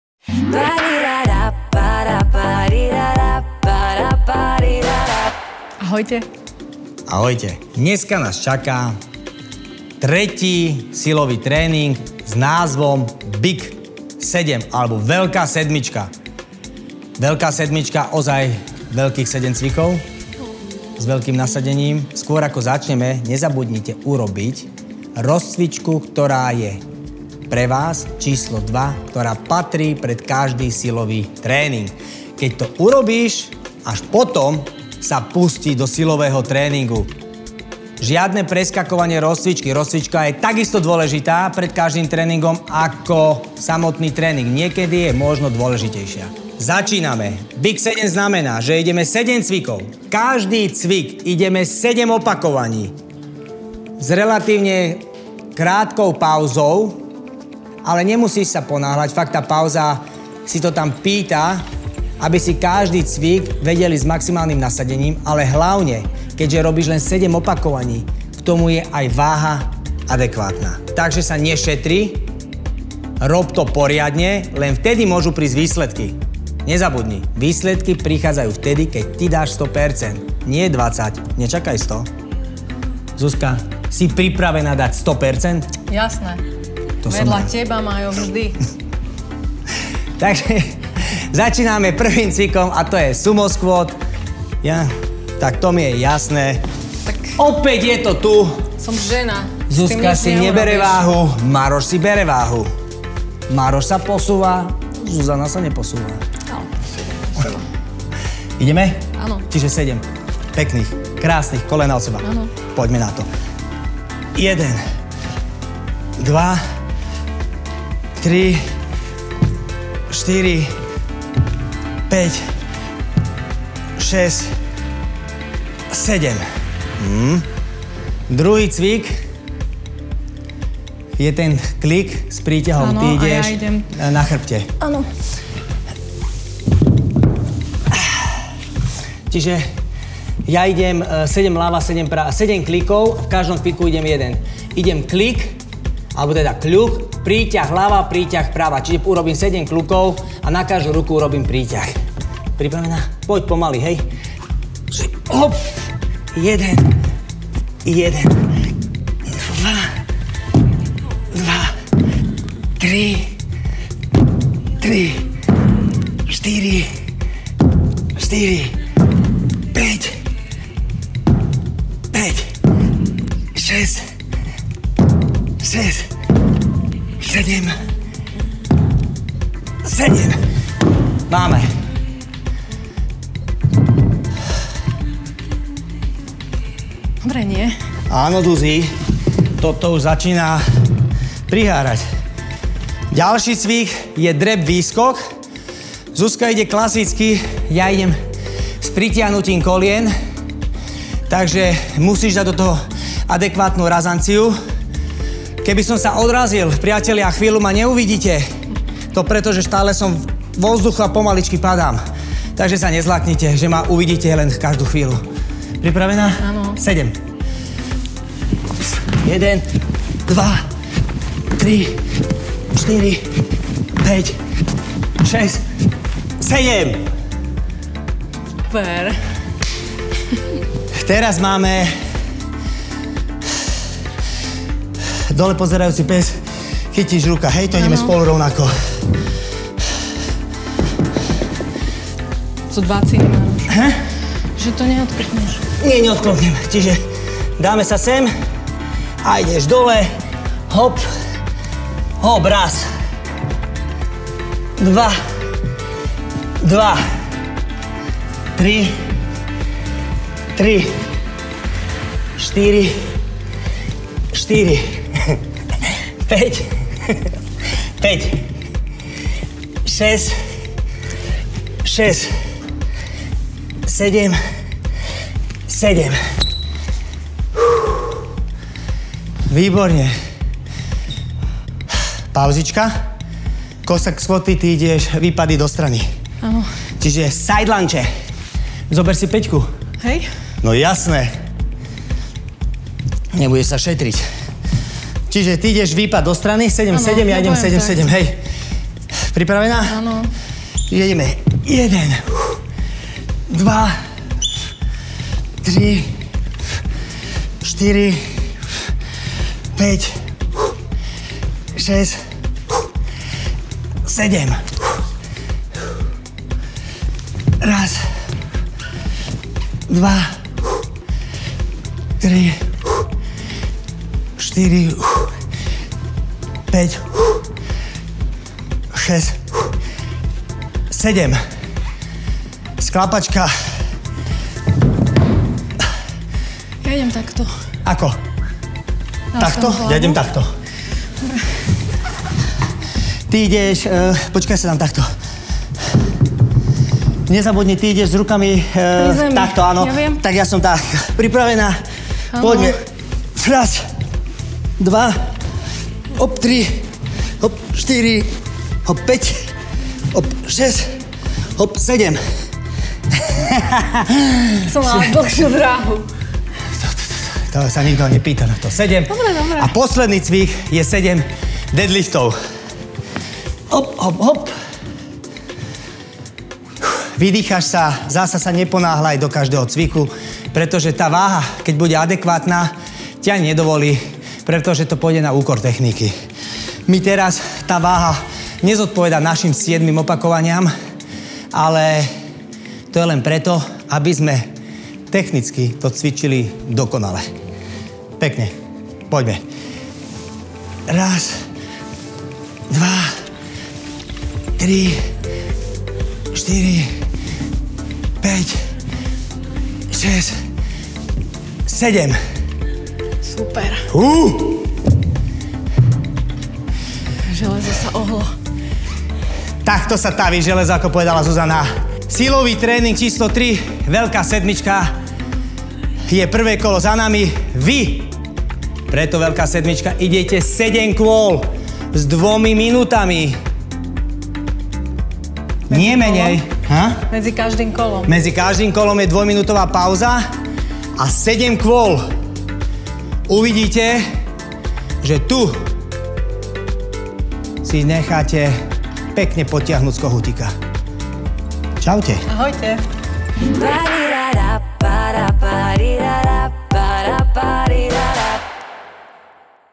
Silový tréning: Veľká sedmička
Tento tréning pozostáva zo 7 zložených cvičení. To znamená, že počas jedného cviku zaťažíme viac svalových partií, a tým spálime omnoho viac kalórií. Počas tréningu je zvýšená tepová frekvencia. Zložené cvičenia sú náročnejšie na techniku, a preto aj koncentrácia je omnoho vyššia. Účinnosť tohto tréningu je vysoká. Váha pri jednotlivých cvičeniach by mala byť nastavená tak, aby ste zvládli aj 9 opakovaní. Medzi cvikmi je pauza 30 sekúnd až 1 minúta, po odcvičenom kole je pauza 2 minúty.

👉🏻Klikajte na a objavte desiatky inšpiratívnych videí našich trénerov.
👉🏻Stiahnite si našu aplikáciu VšZP a vyhrajte športové hodinky GARMIN VENUS SQ, viac informácií nájdete tu